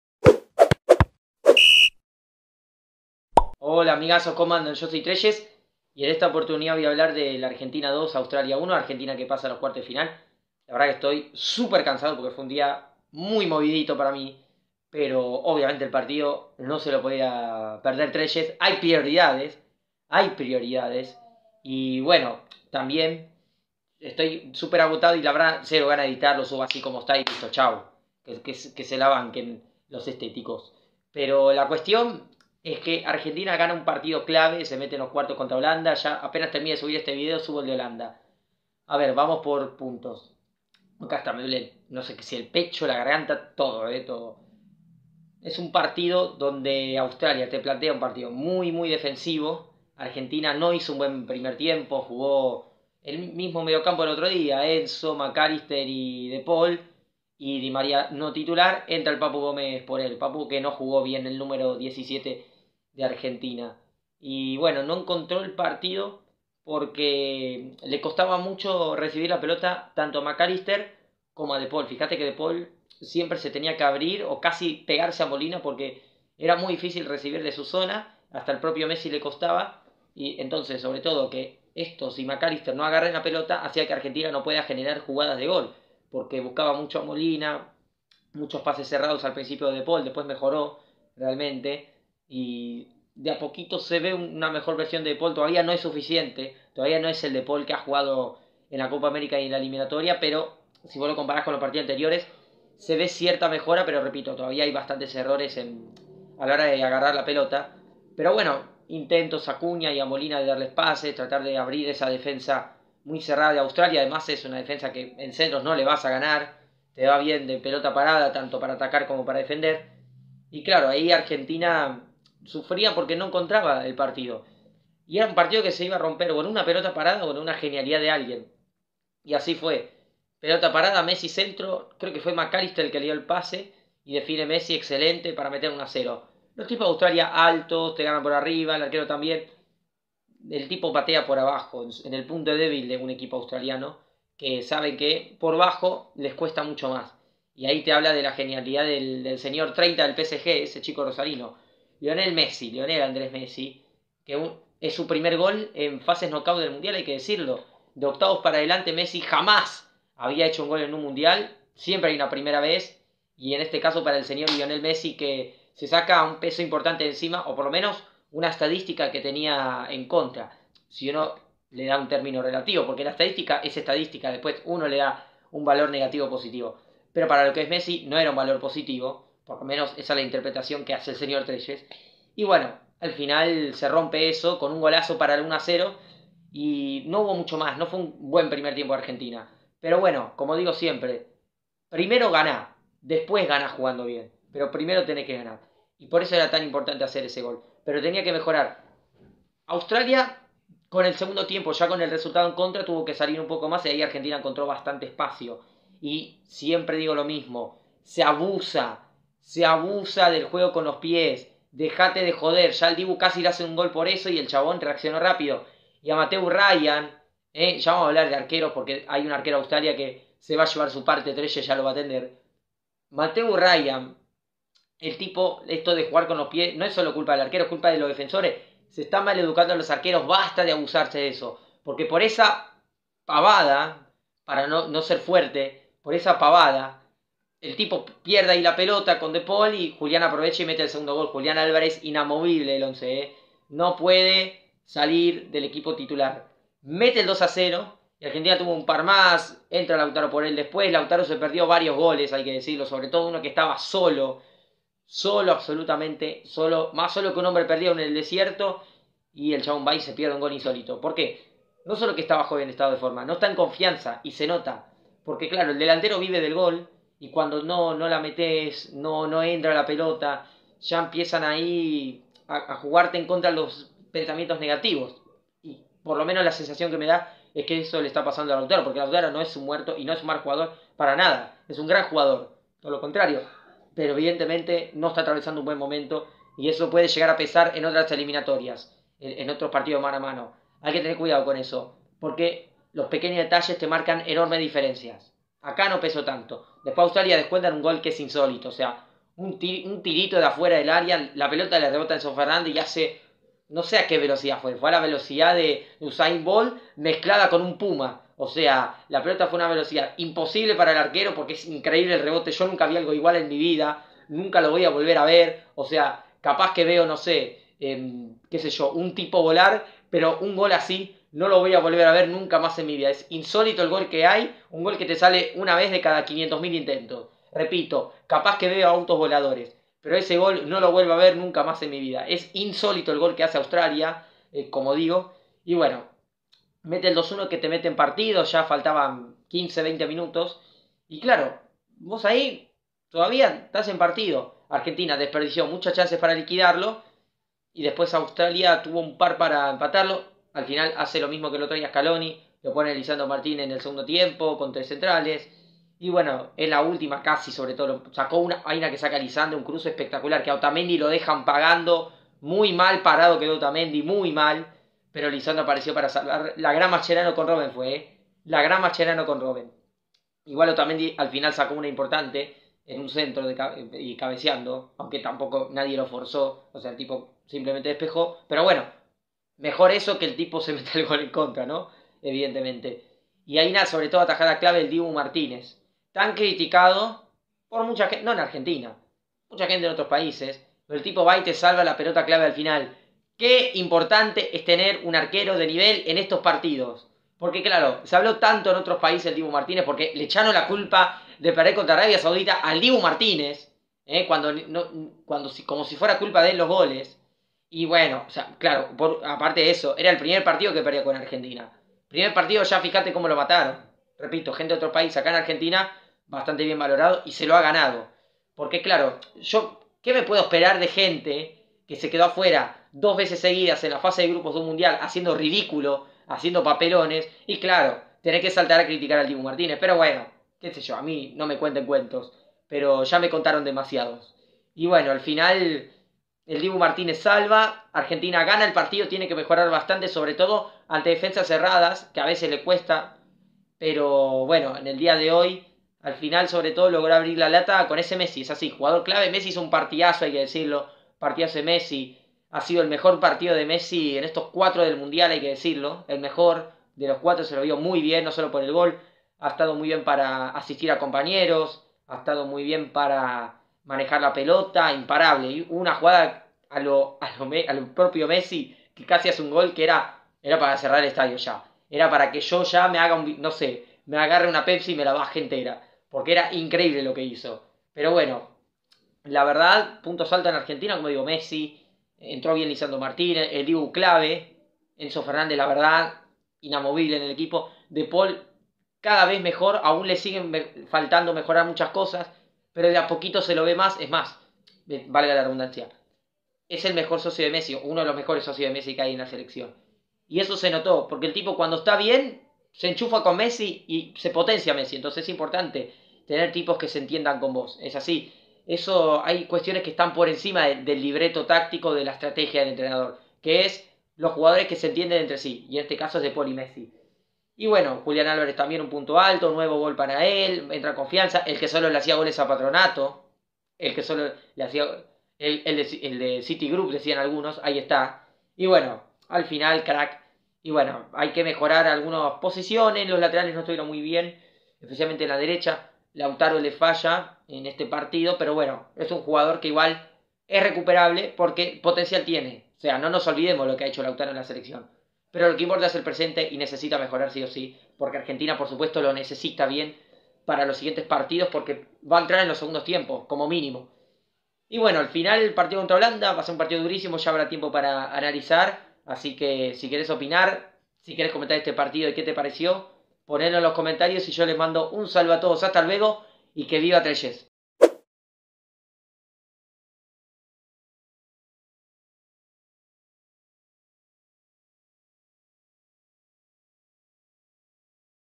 ARGENTINA 2-1 AUSTRALIA / MI OPINIÓN Y ANÁLISIS
Argentina le ganó 2 a 1 a la Australia de Graham Arnold y se clasificó a los cuartos de final del Mundial.